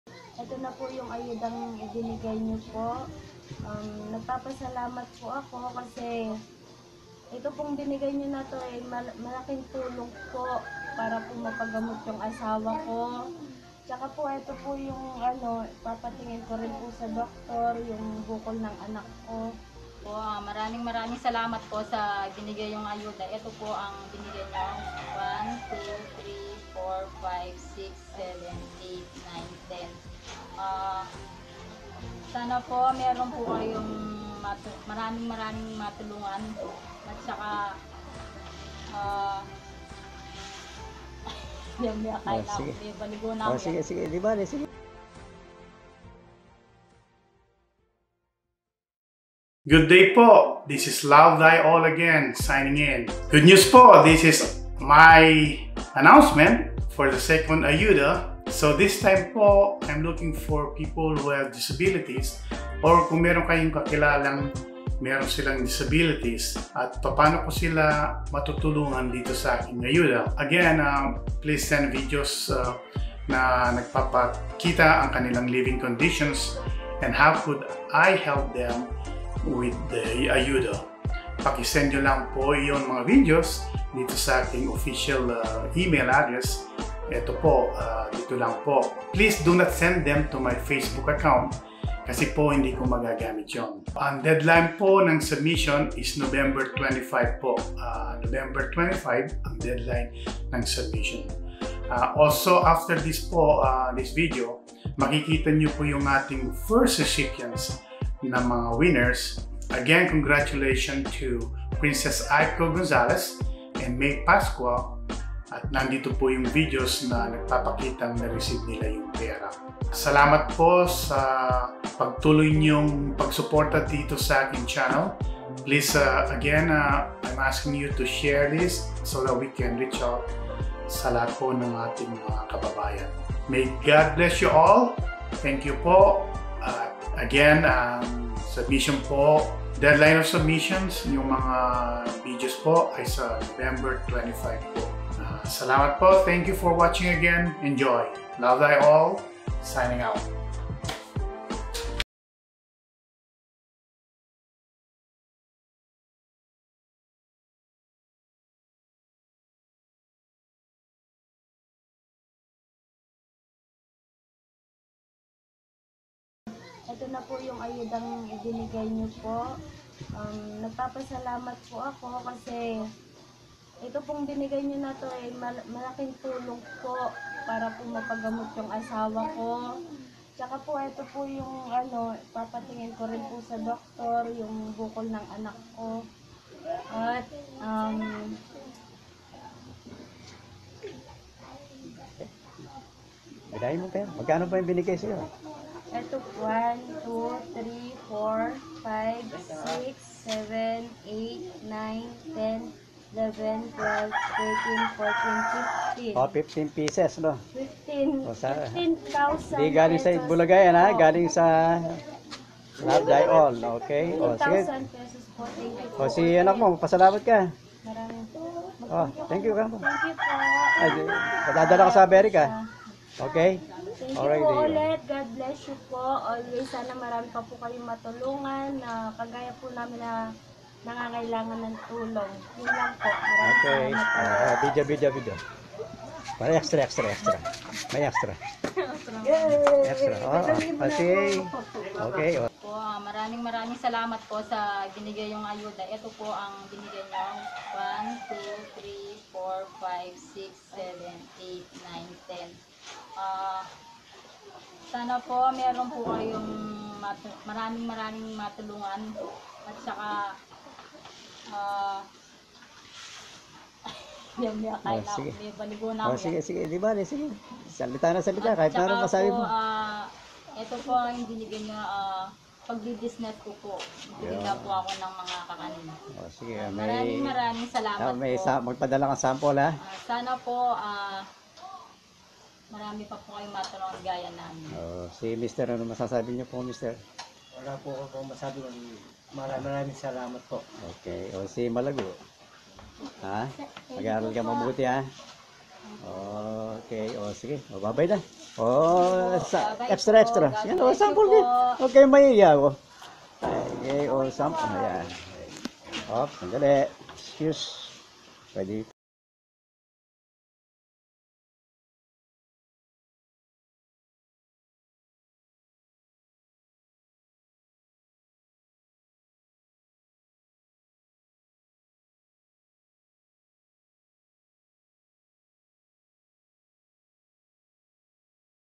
Second Ayuda Announcement
Looking for people to help with Ayuda!  Show me how to help and provide Ayuda to those who are truly deserving.